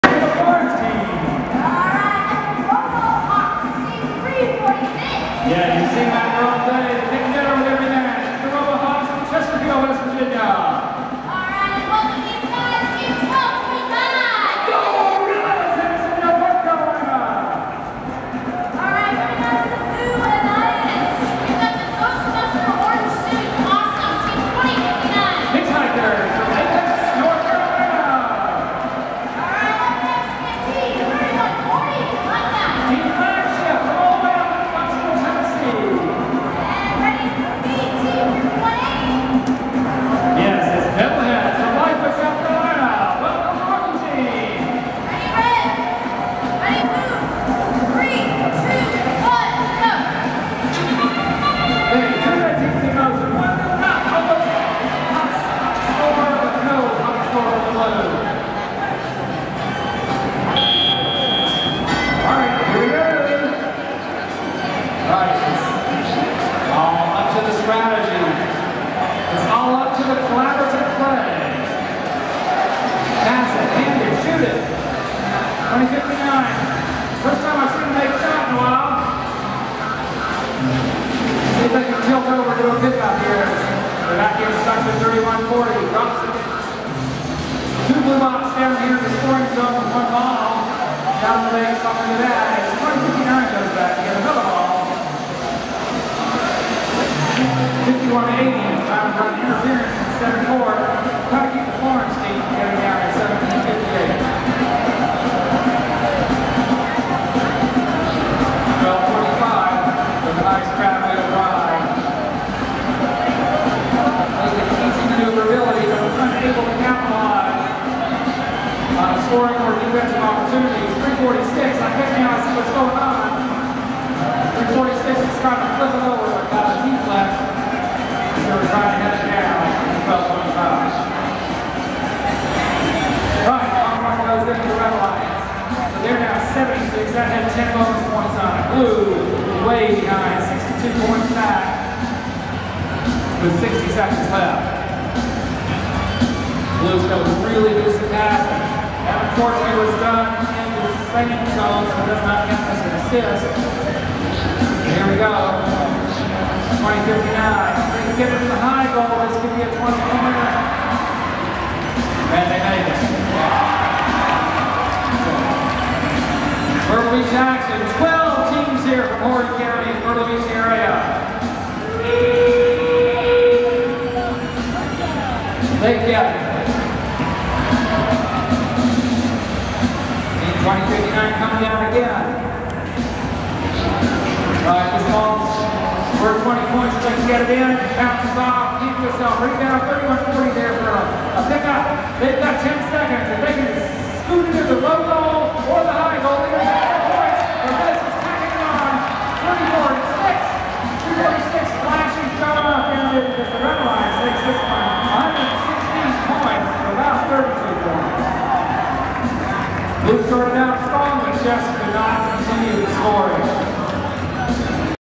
Team 1758 Technomancers on 2014 Palmetto Regional Quals 57
2014 FIRST Robotics Palmetto Regional Competition, Qualifications 57 in Myrtle Beach, South Carolina.